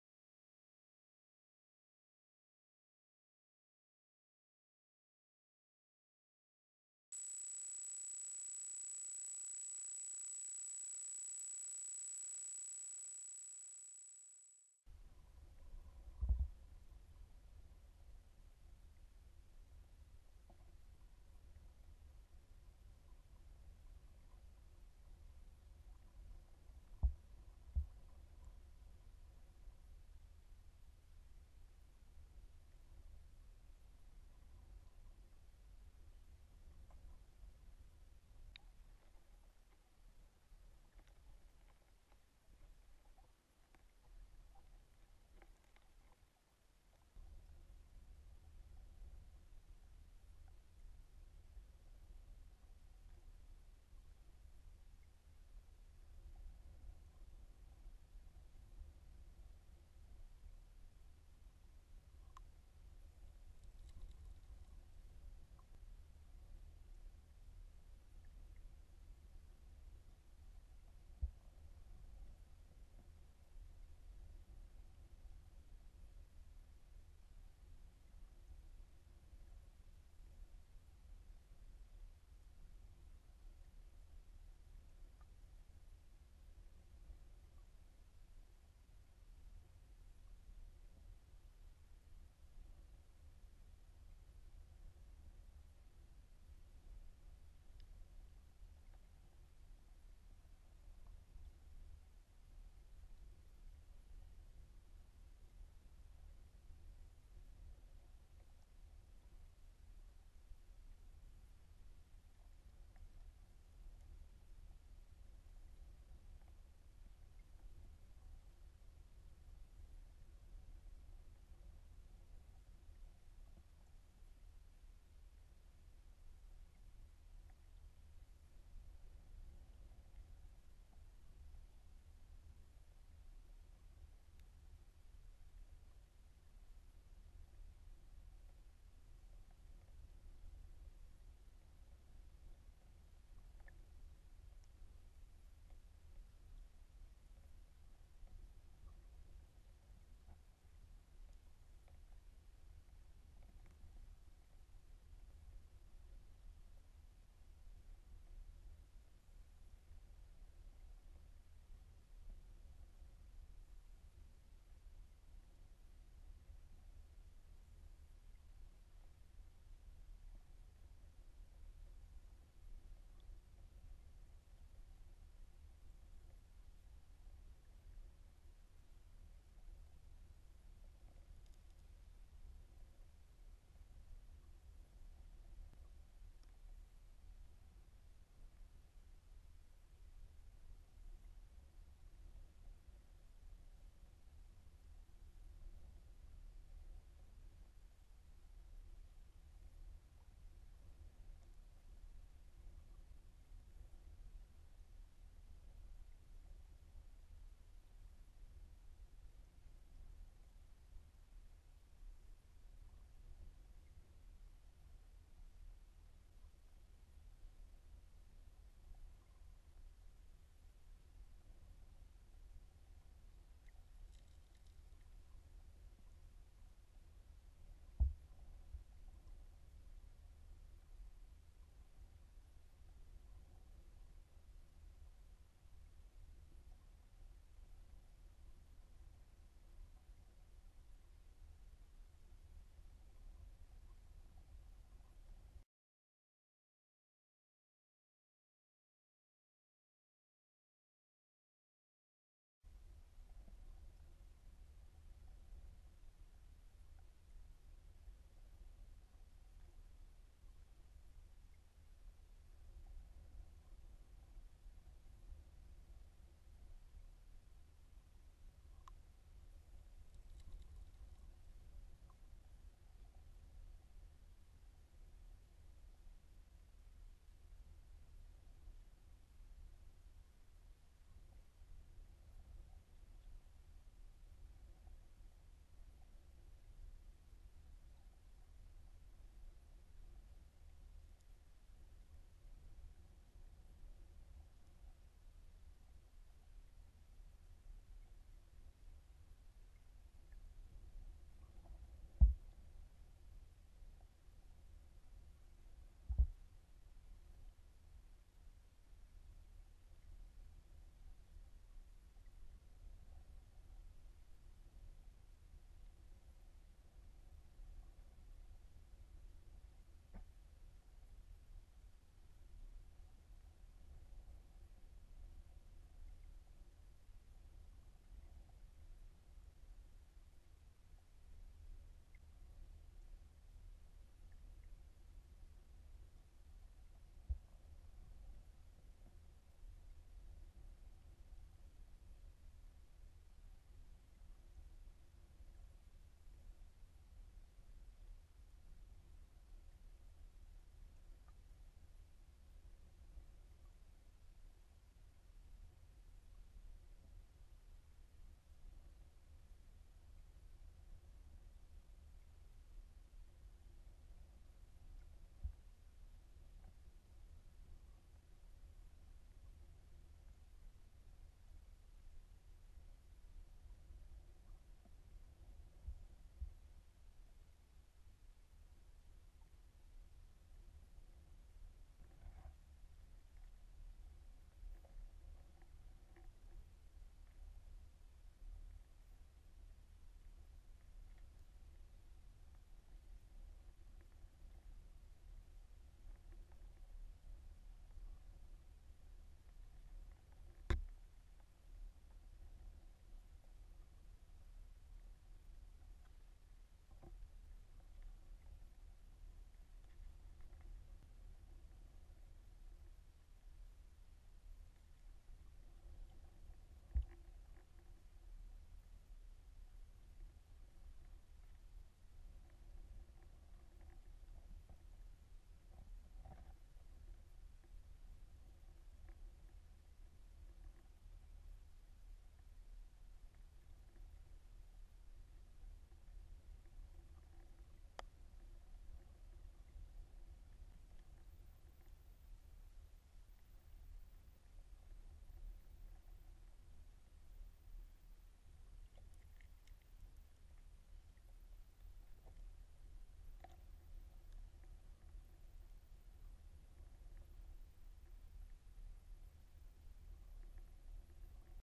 Bush-Cam 1/12/2013
Short Video of Atlantic Salmon at spawning time, It is still quite early and in a few weeks time I hope to get some better shots! sorry about the photo in the middle of the video small error while editing!